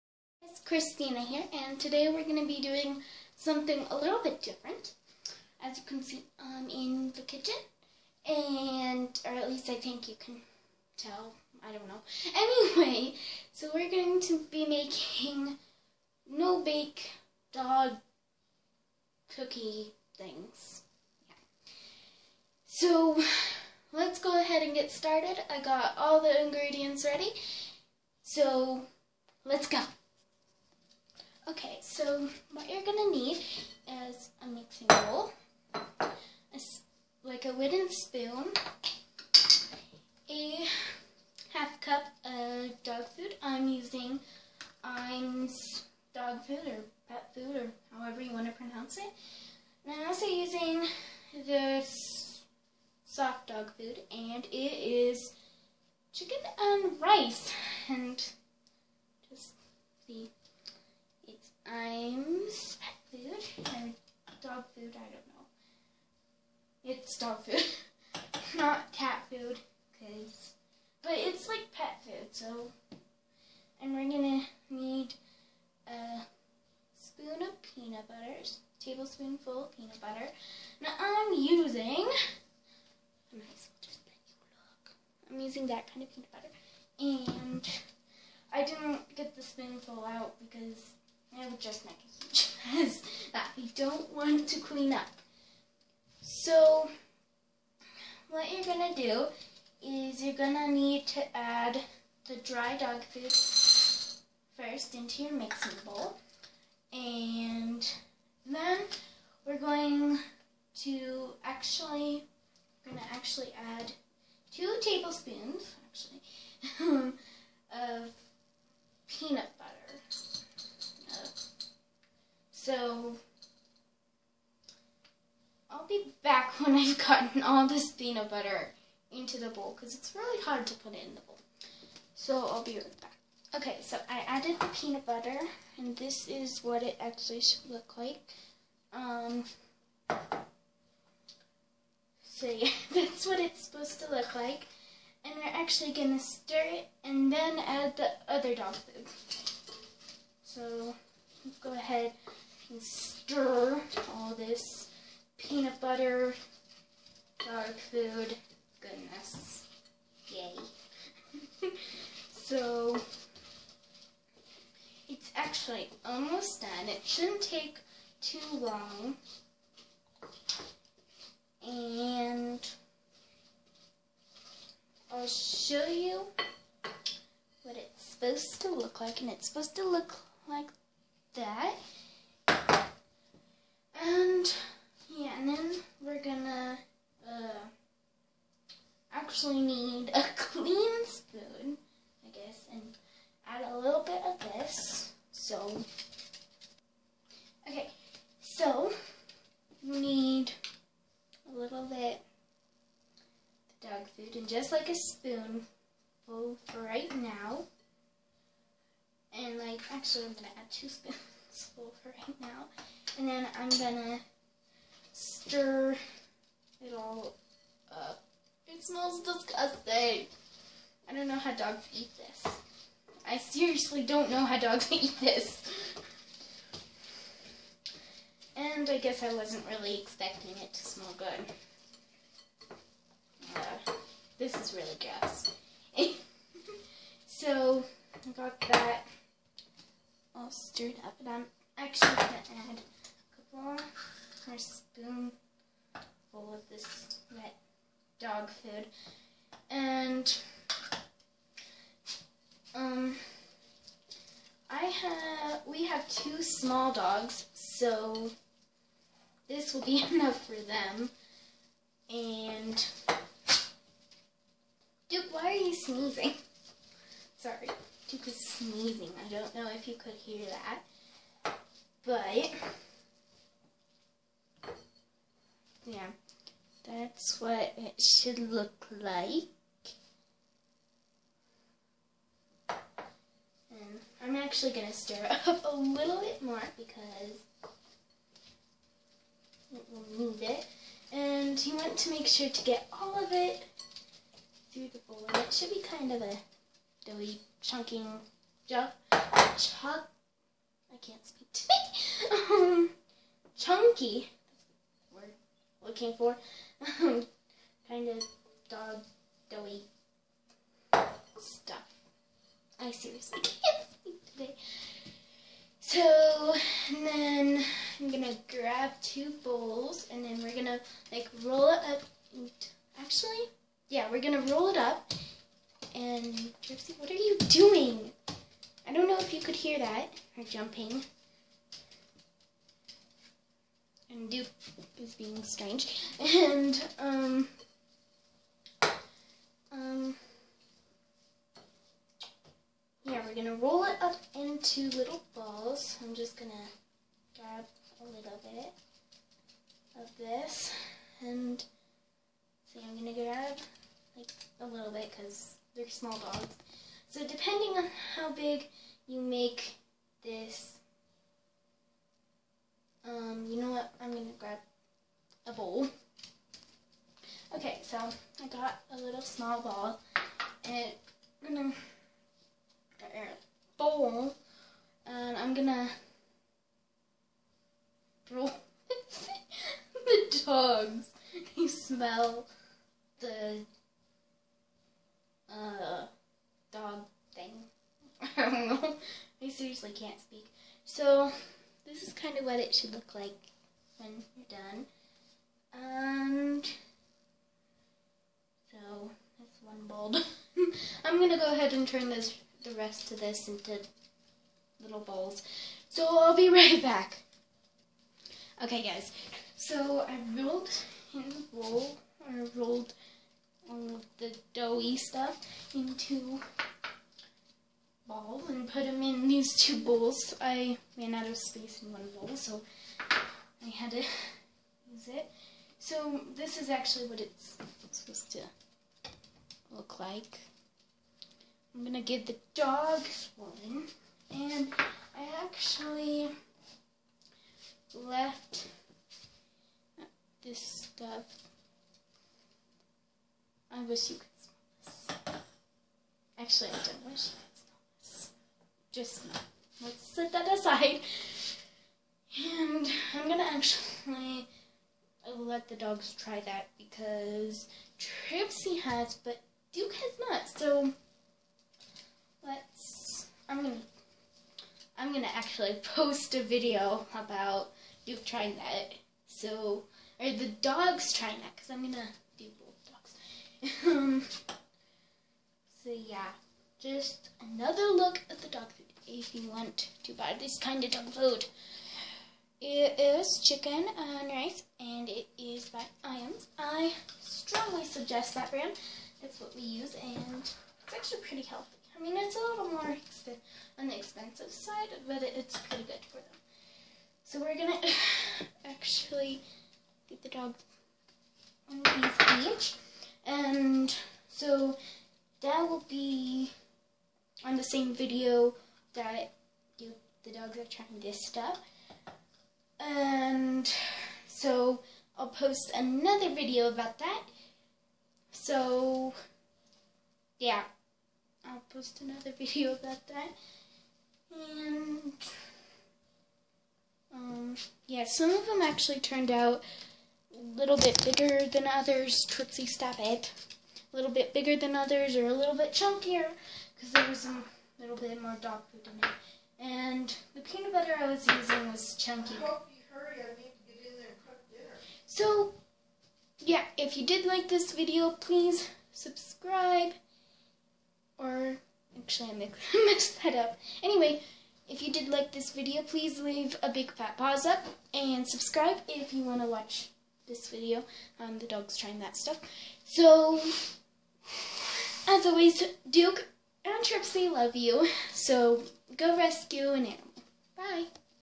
Peanut butter dog cookies! With Kristina!
Today I thought I would try something new and cook for my dog's Duke Dog and Tripsy this is to make up for not posting videos in a while and for not recording Duke Dogs birthday party 🐶